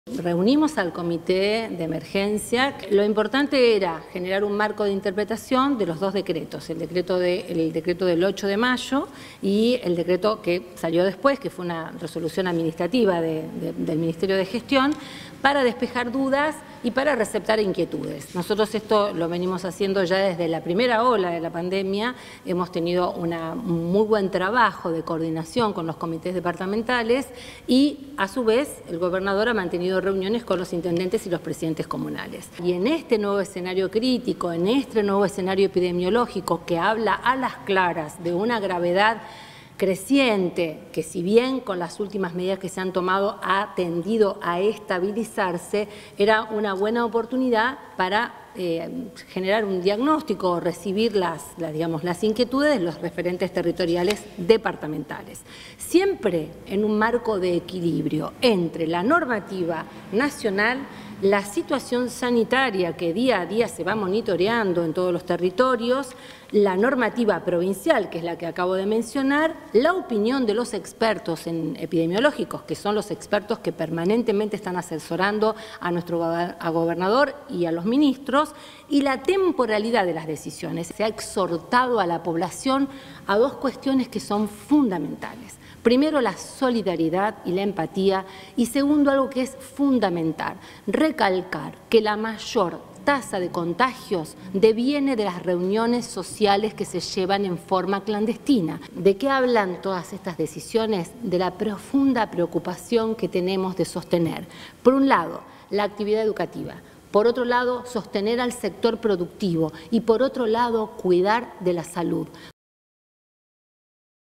CORACH: “NO PODEMOS HACER DE CUENTA QUE NO PASA NADA; LA PANDEMIA SE COBRA VIDAS TODOS LOS DÍAS”
Lo aseguró el ministro de Gestión Pública luego de encabezar una mesa de trabajo con intendentes y presidentes comunales para analizar las medidas vigentes tras el último decreto provincial.